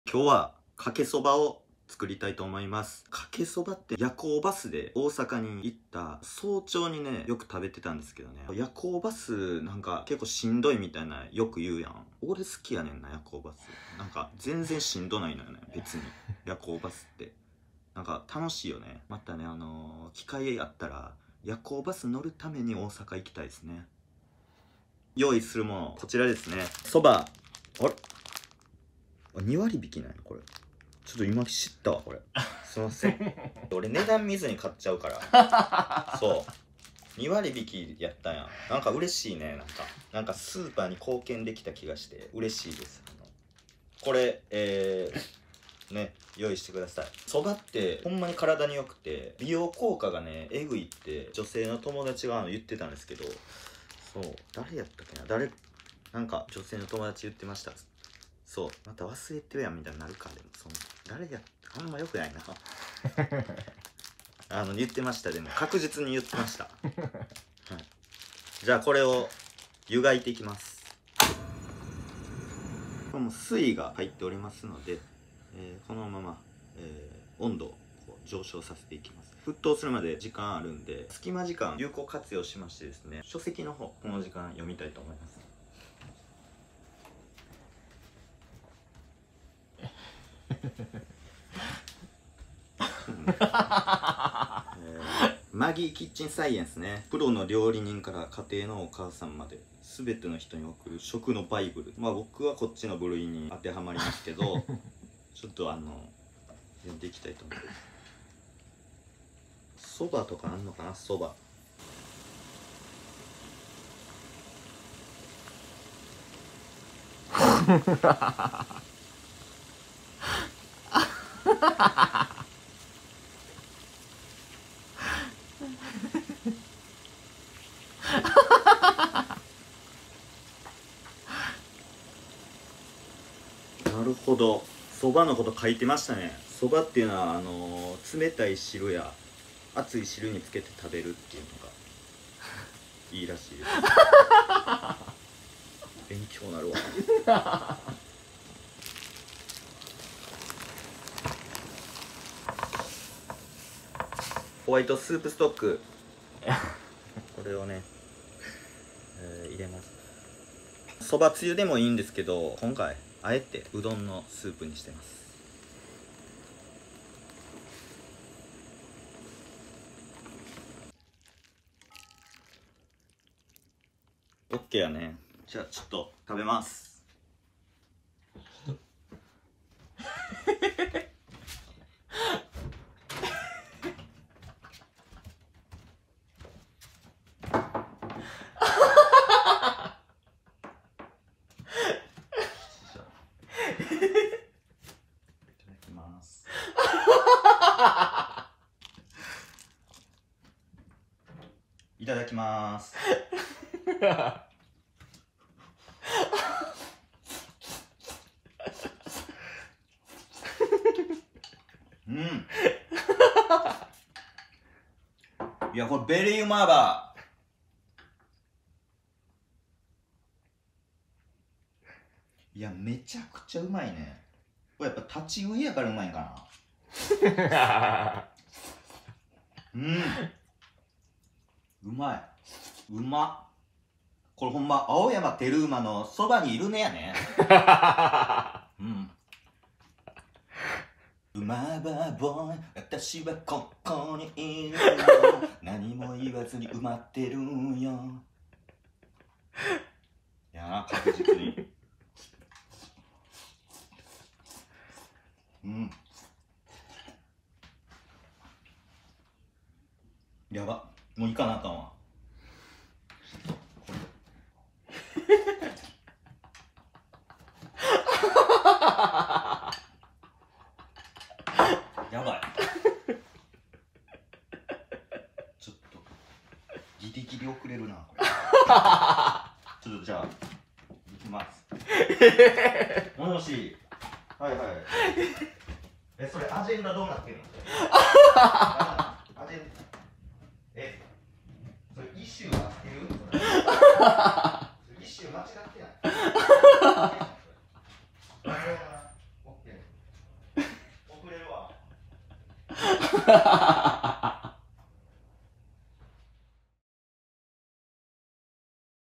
自宅で立ち食いそばをしてかっこつける港区家賃3万7千円男
▽密着されている人：岡田